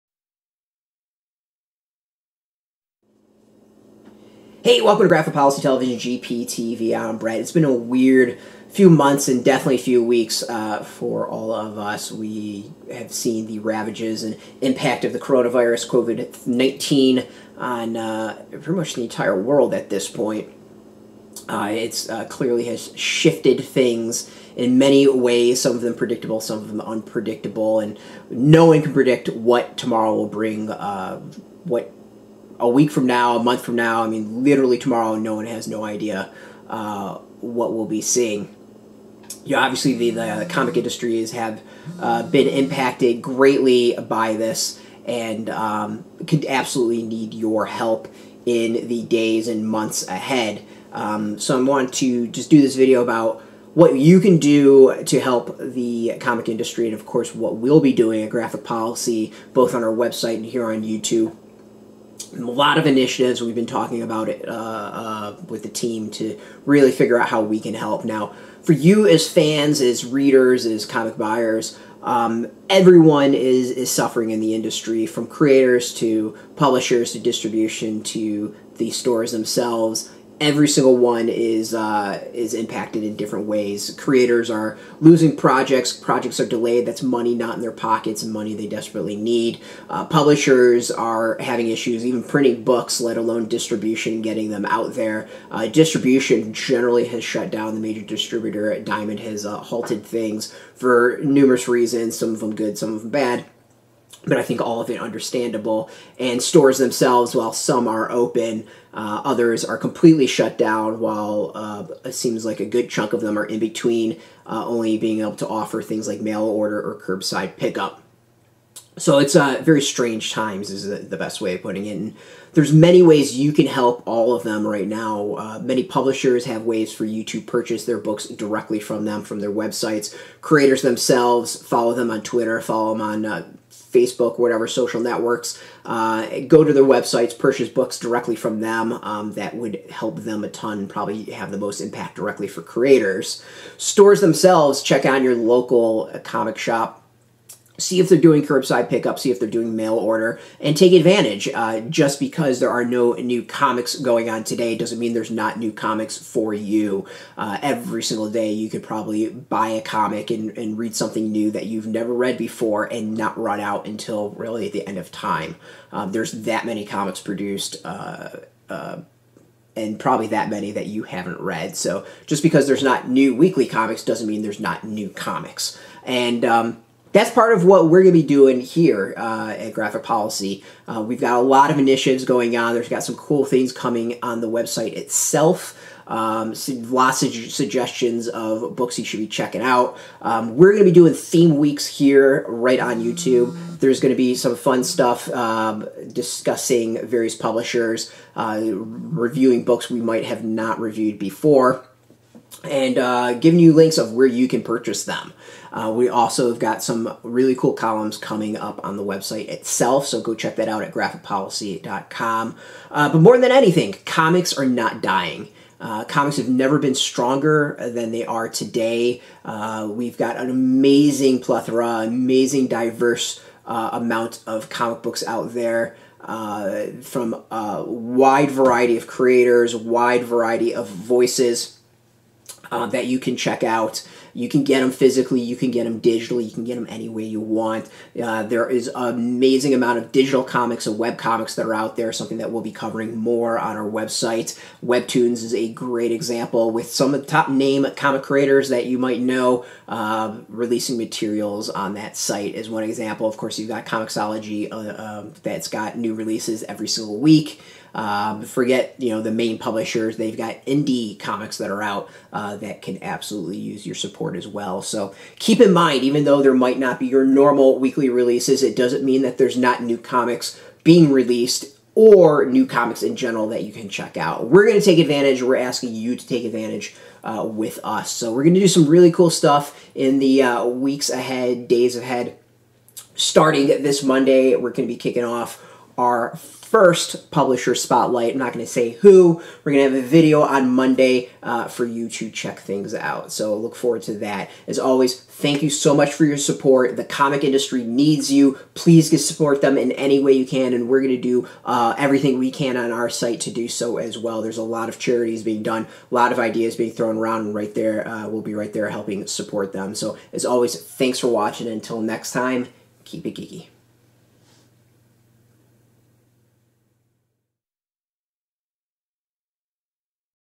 A Special Message from GPTV... What's Next?
We're currently in weird times and Graphic Policy's founder has a special message for our viewers and readers.

While the "new" comics might be dwindling that doesn't mean we don't have a lot coming your way...